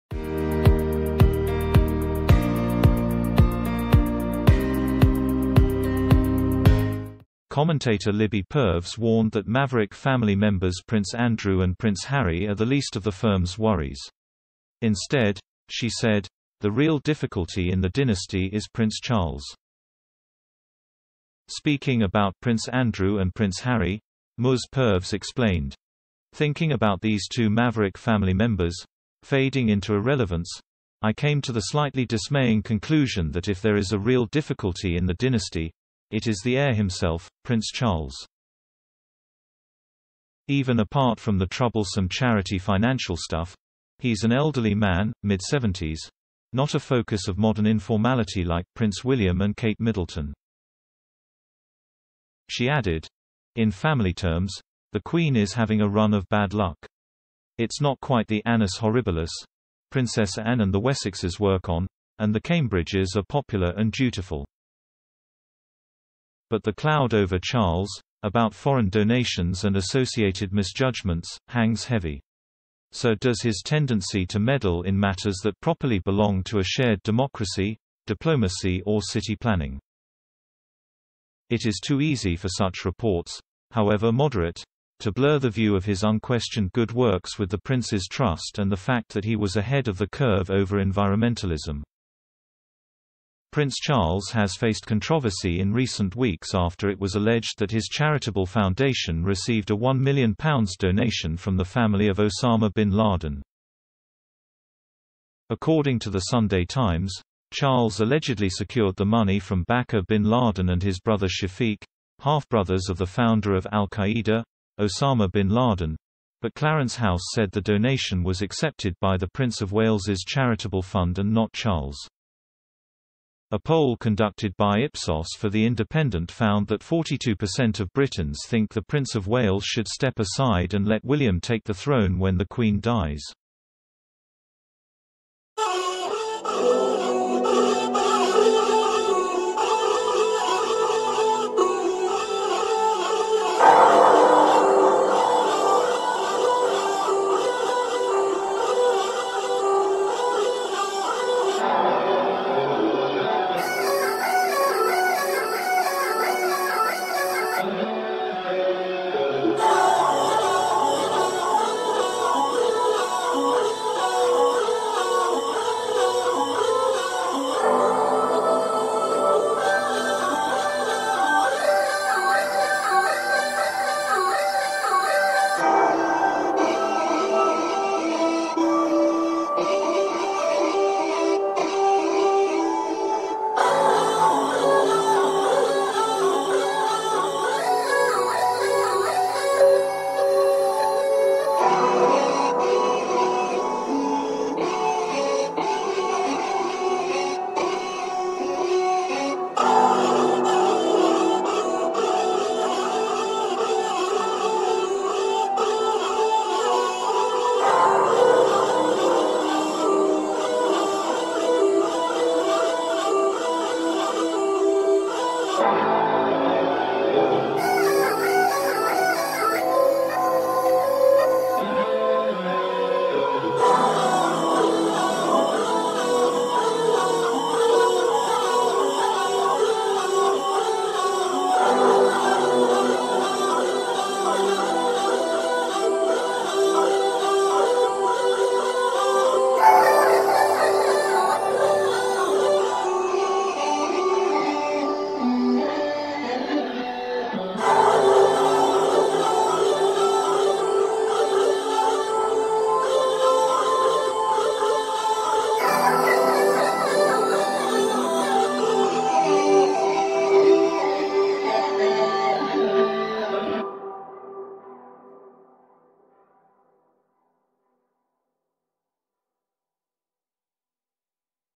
Royal Family warned looming threat could tear monarchy apart: 'Difficulty in the dynasty'
Commentator Libby Purves warned that "maverick family members" Prince Andrew and Prince Harry are the least of the Firm's worries.

Instead, she said, the "real difficulty in the dynasty" is Prince Charles. Speaking about Prince Andrew and Prince Harry, Ms Purves explained: "Thinking about these two maverick family members, fading into irrelevance, I came to the slightly dismaying conclusion that if there is a real difficulty in the dynasty, it is the heir himself, Prince Charles.